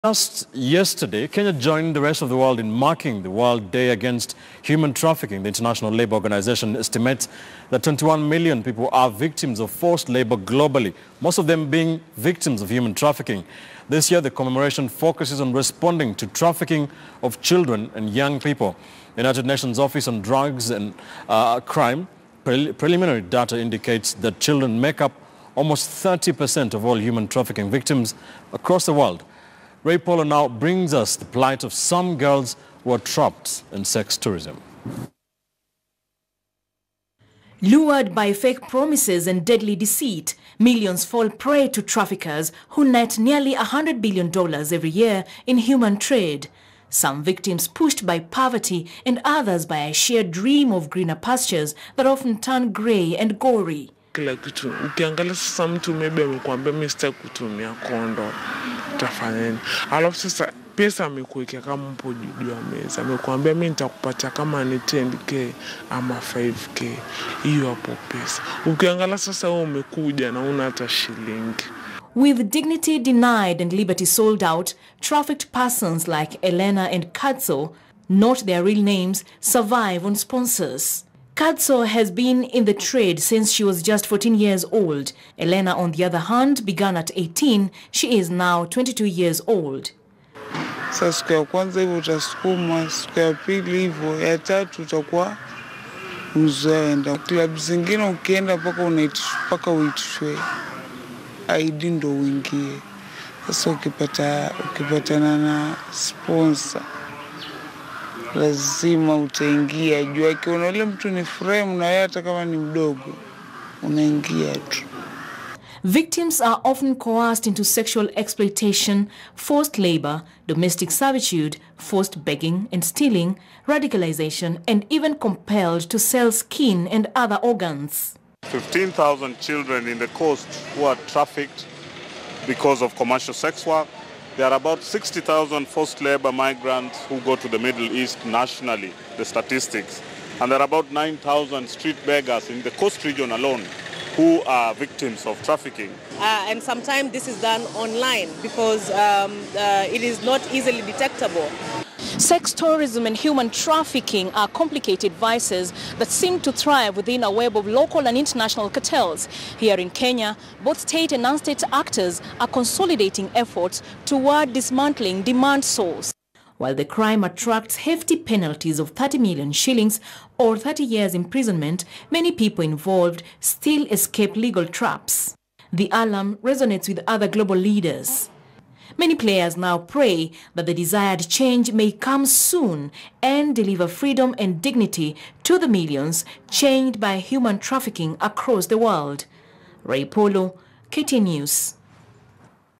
Forms of Human Trafficking in the world today | KTN News Desk
Kenya joined the rest of the world in marking world day against human trafficking. The international labour organization estimates that 21 million people are victims of forced labour globally, most of them being victims of human trafficking. This year, the commemoration focuses on responding to trafficking of children and young people. The United Nations office on drugs and crime (UNODC) preliminary data indicates that children make up almost 30 per cent of all human trafficking victims worldwide.